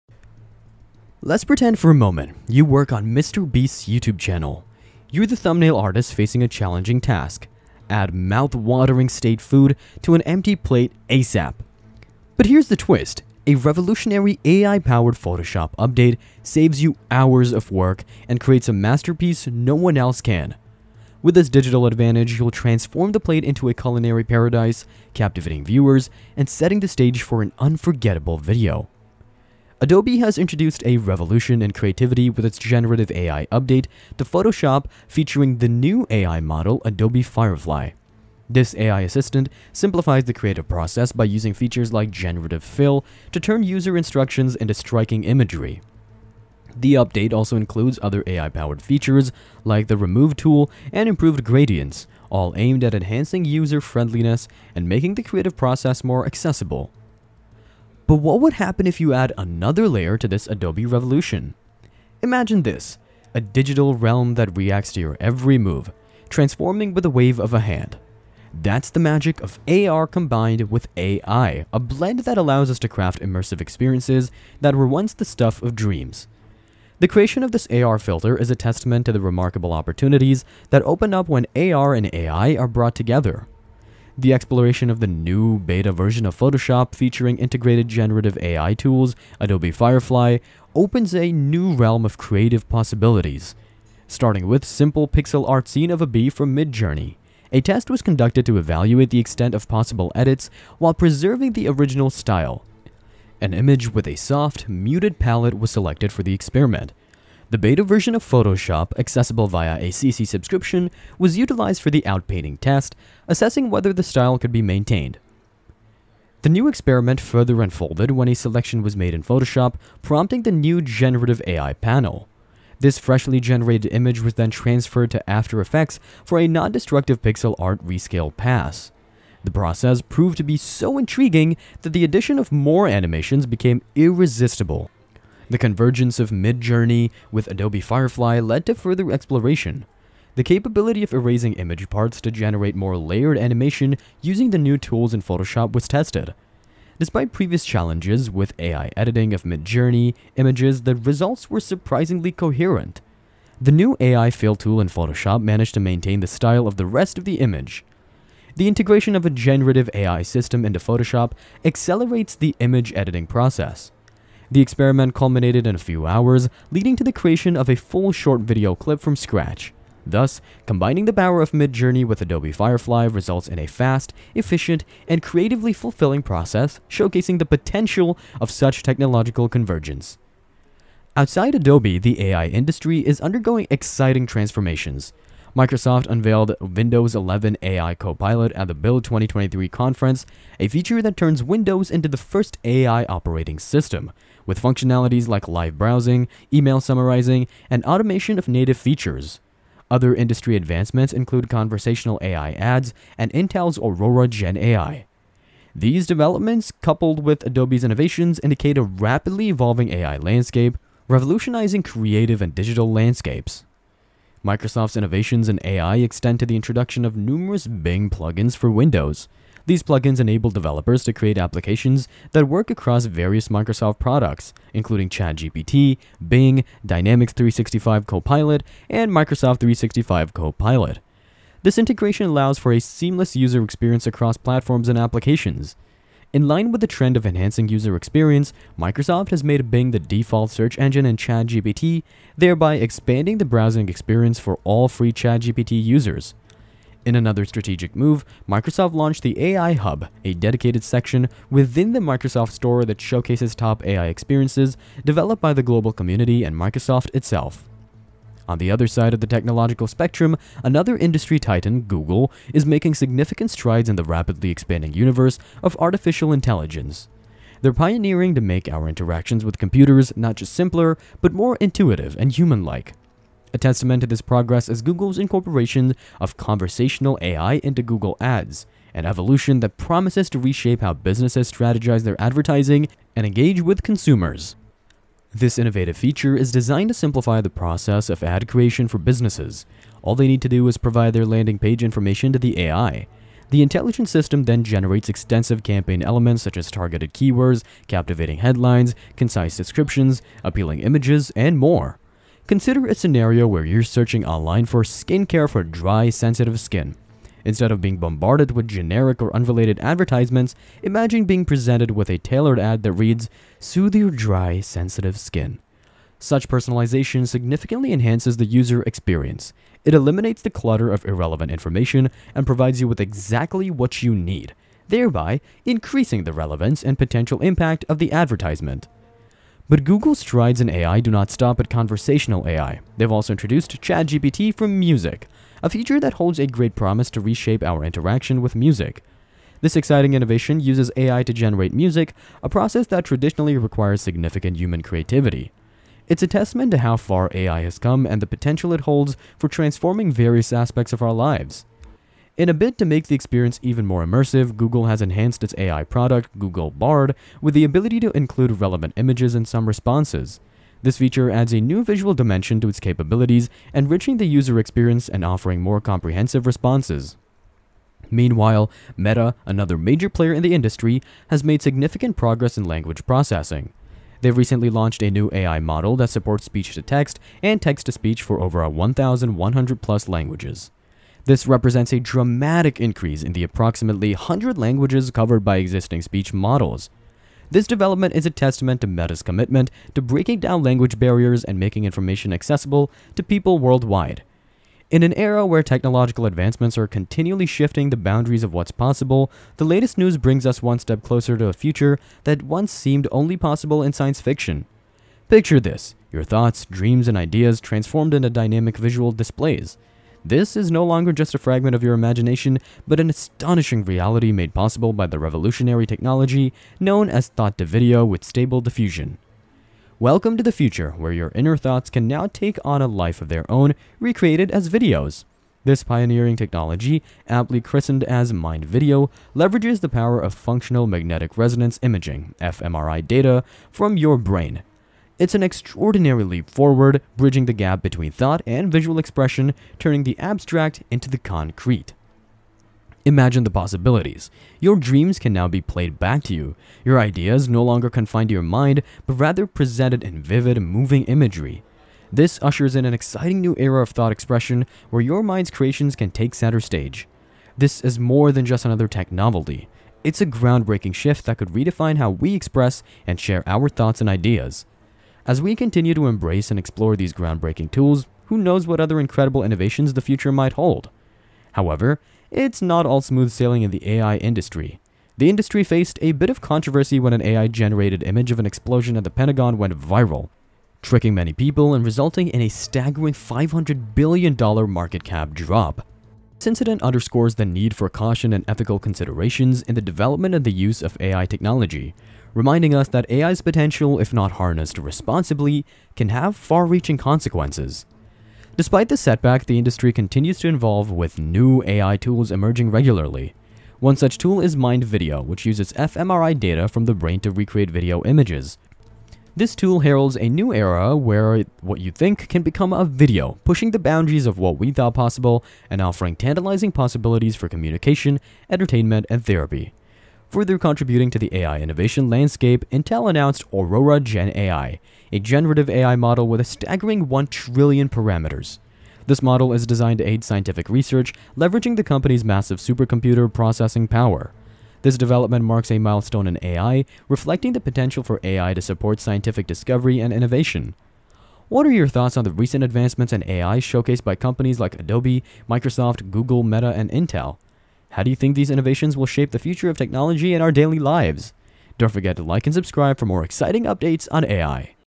Google's NEW AI, Adobe Shocks Entire Industry, Window Powered By AI & More! (Rapid Breakdown)
AI news this week has been crazyyy! Brace yourself for a mind-blowing journey into a world where technology seamlessly integrates with our creativity, transforms our interactions, and unlocks endless possibilities.